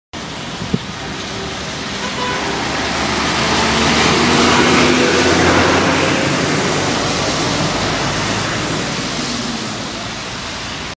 Street Corner in Shanghai at Night
Asia is a big fan of skyscraper light shows.